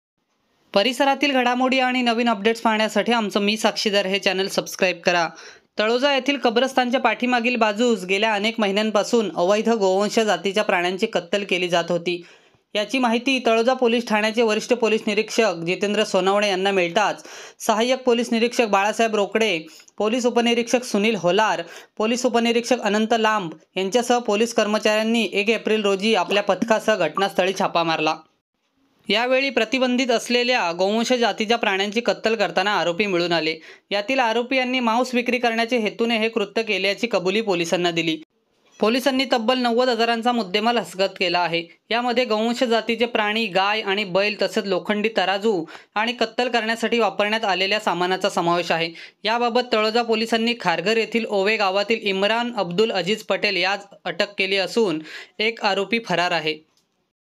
तळोजातील कत्तलखान्यात पोलिसांचा छापा, कत्तल करताना केली एकाला अटक, 1 फरार
तळोजातील कत्तलखान्यात पोलिसांचा छापा,कत्तल करताना केली एकाला अटक, एक फरार#कत्तलखानातळोजा ओवे कत्तलखाना kattalkhana अटक ओवा पेठ#